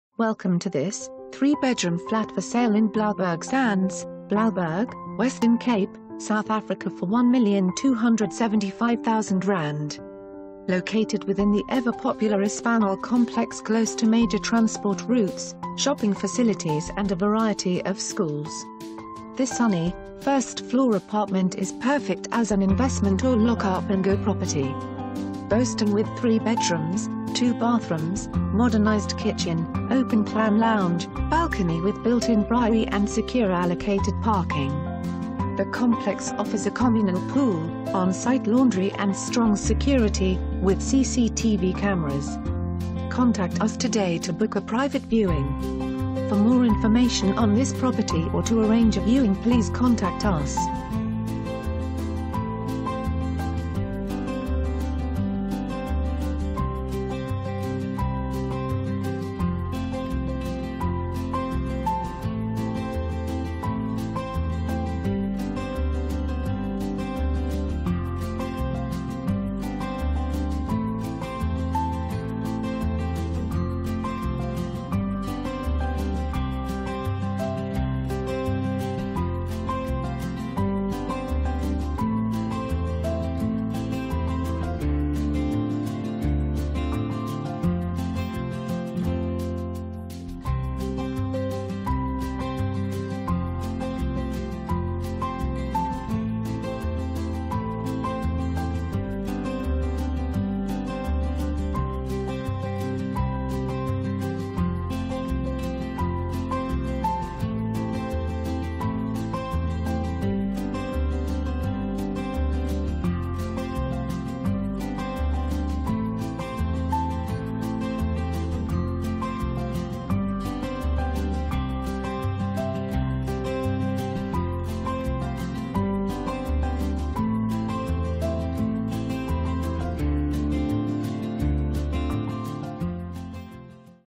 3 Bedroom Flat For Sale in Blouberg Sands, Blouberg, Western Cape, South Africa for ZAR 1,275,000
Located within the ever popular ?Espanol? complex: Close to major transport routes, shopping facilities and a variety of schools.

This sunny, first floor apartment is perfect as an investment or lock-up & go property!

Boasting with 3 Bedrooms, 2 Bathrooms, Modernised kitchen, Open plan lounge, Balcony with built-in braai an...